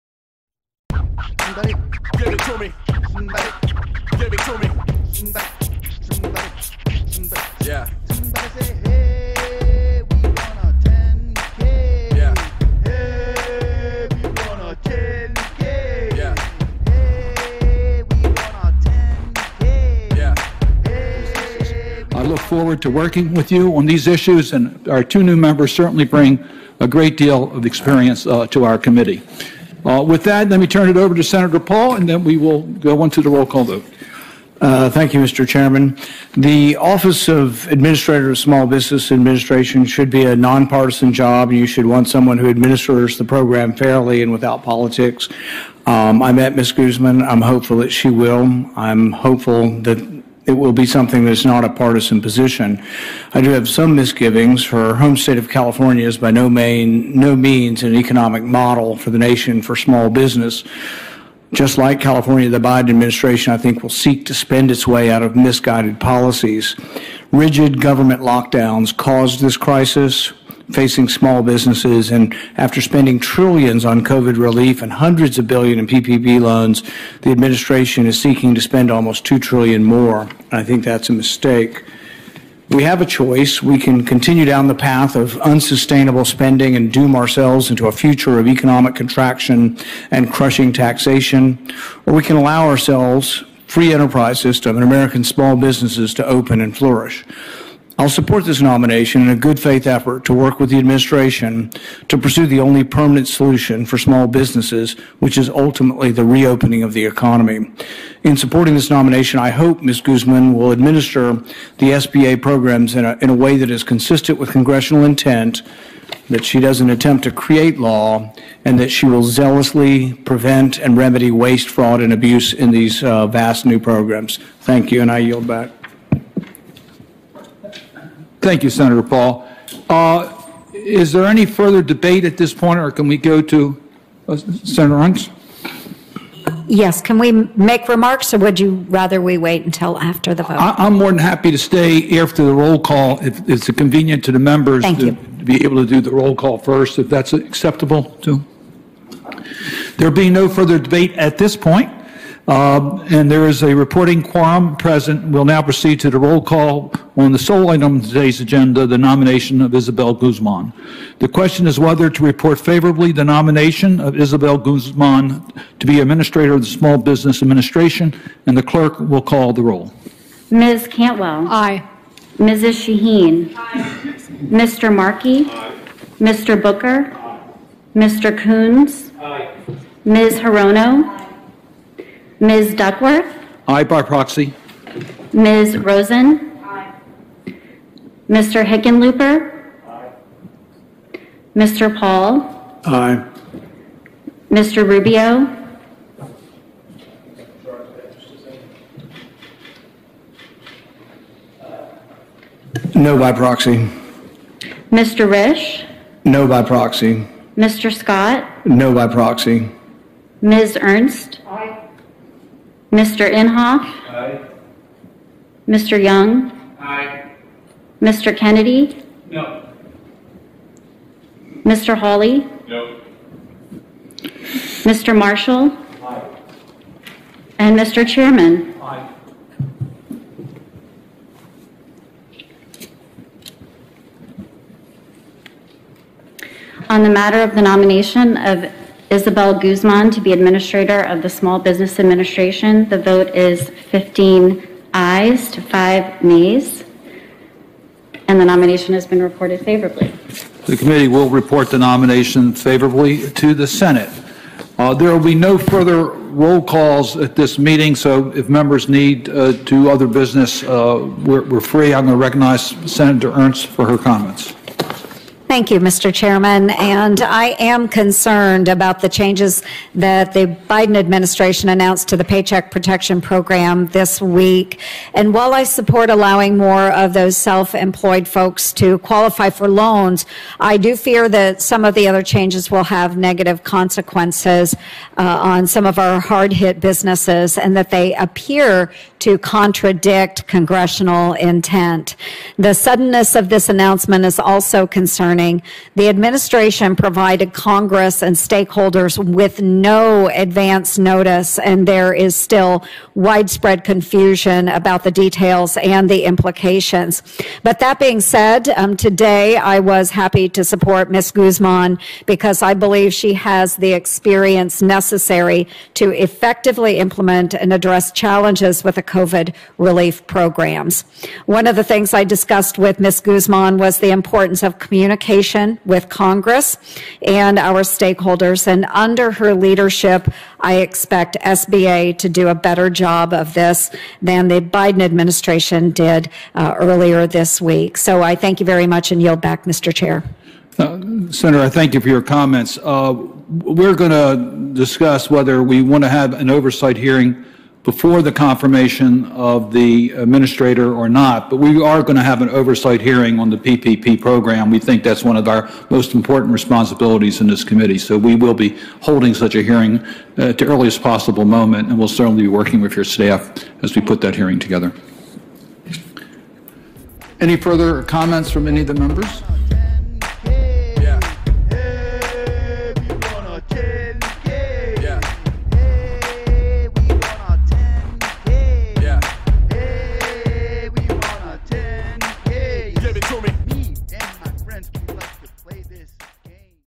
Isabel Guzman Role Call vote Nomination for SBA Administrator 2/24/2021 speaknsee
Isabel Guzman's Nomination today for the  SBA Administrator position.

 speaknsee